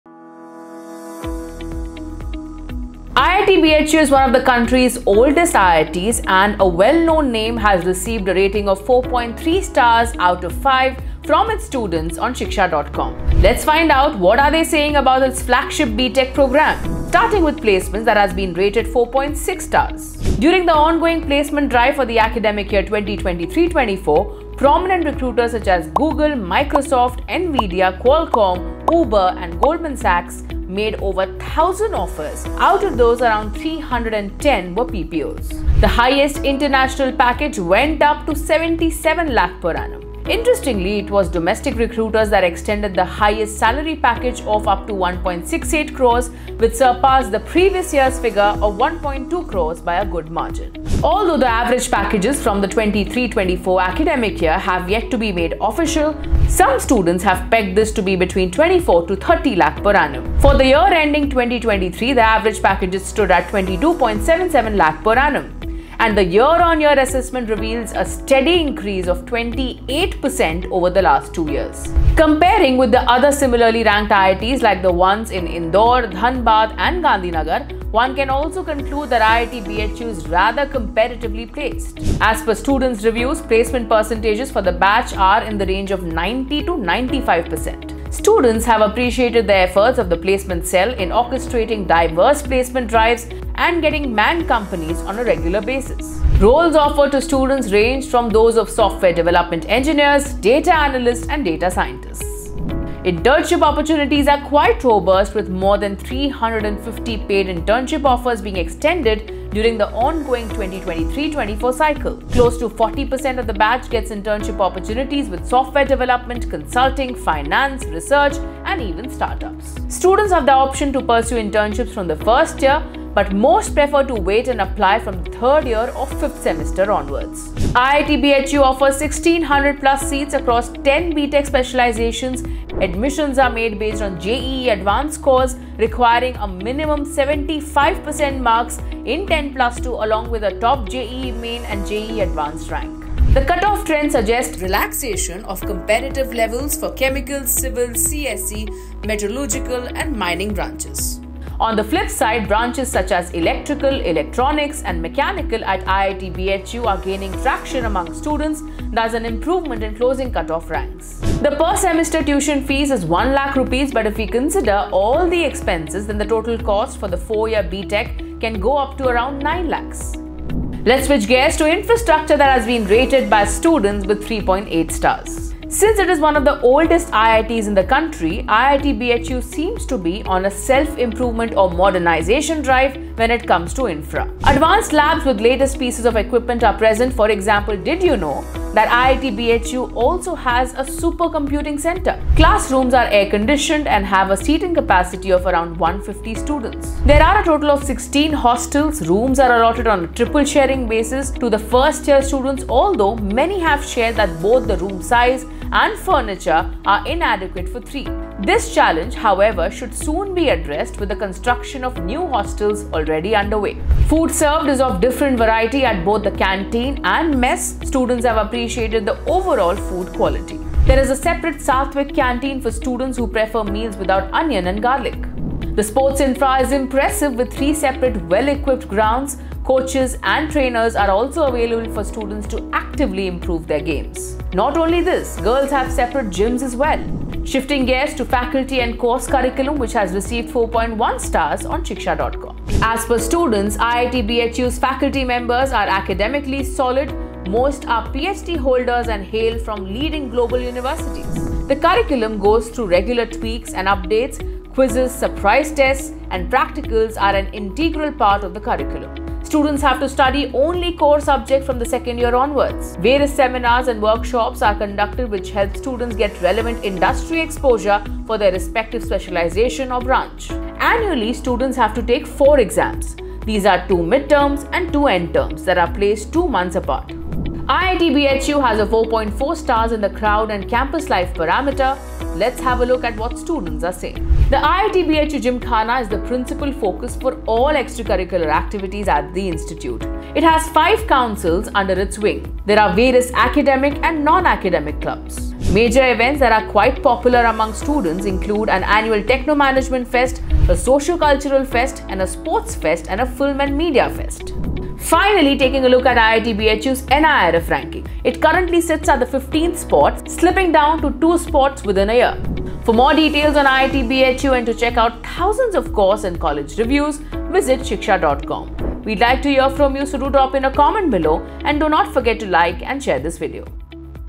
IIT BHU Varanasi (IIT BHU) Review : Placements, Cut Off, Courses, Admission 2024, Ranking
IIT BHU is one of the most prominent Engineering institutes in India. IIT BHU is ranked 15th among the top Engineering Institutions in NIRF Ranking 2023. IIT Varanasi is approved by the UGC and the All India Council for Technical Education (AICTE). IIT BHU offers various Undergraduate, Postgraduate, and PhD programmes under 15 departments and 3 inter-disciplinary schools. IIT BHU courses include BTech, BPharm, BArch, MTech, MPharm, MSc, PhD, Integrated MTech (IMD), and Integrated Dual Degree (IDD).

Watch this in-depth college review that brings you all the essential insights, including the pros and cons of studying at IIT BHU.

0:00- IIT BHU Varanasi Intro
0:17- IIT BHU Varanasi Placements
2:07- IIT BHU Varanasi Courses Offered and Admission Criteria
2:42- IIT BHU Varanasi Course Fees
2:51- IIT BHU Varanasi Infrastructure
3:57- IIT BHU Varanasi Faculty & Curriculum
4:37- IIT BHU Varanasi Campus Life
5:05- IIT BHU Varanasi NIRF Rank